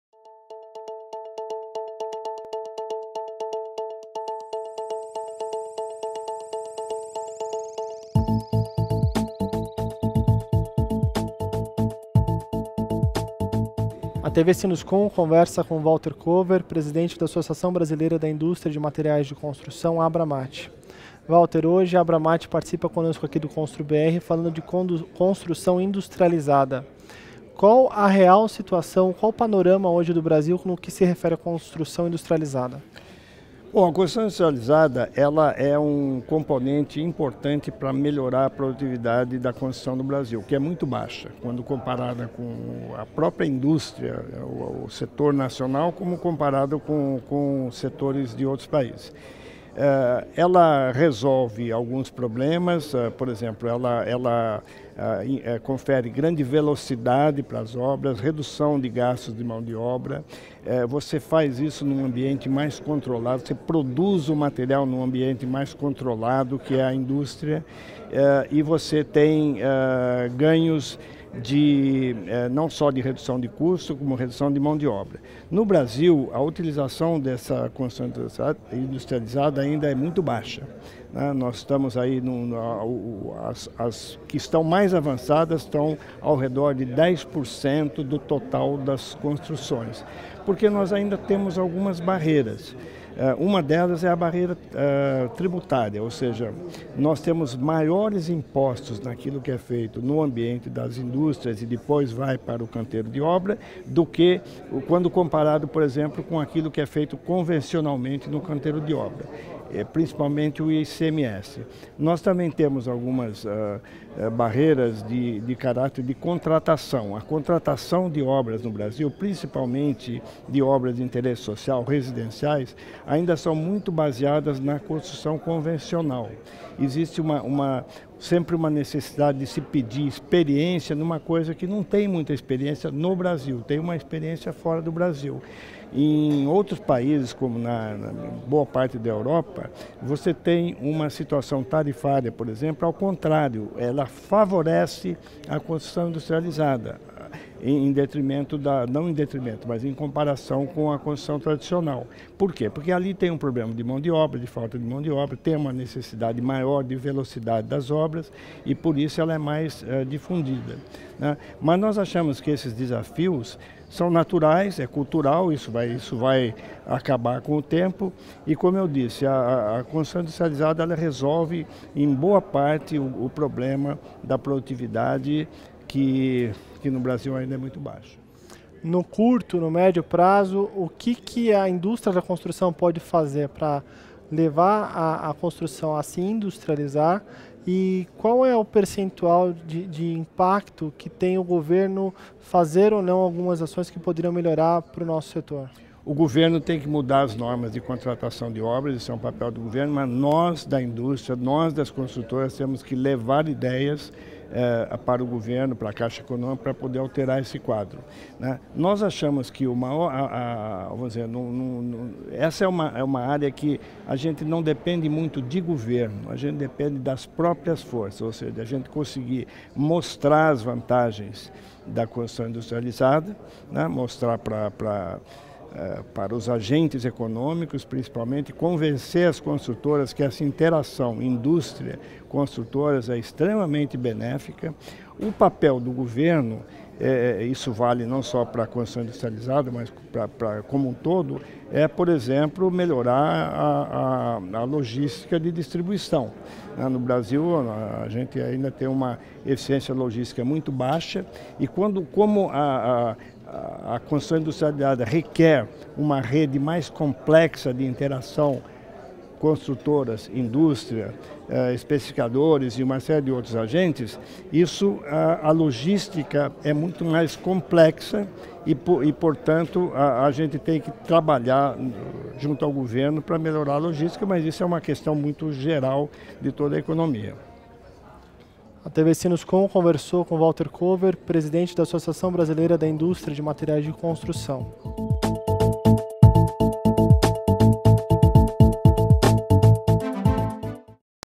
ConstruBR 2016 - Walter Cover - ABRAMAT
O presidente da ABRAMAT, Walter Cover fala sobre a construção industrializada no Brasil, números do setor e os desafios para o aumento da industrialização do setor, ganhos de custo e redução de mão-de-obra na construção.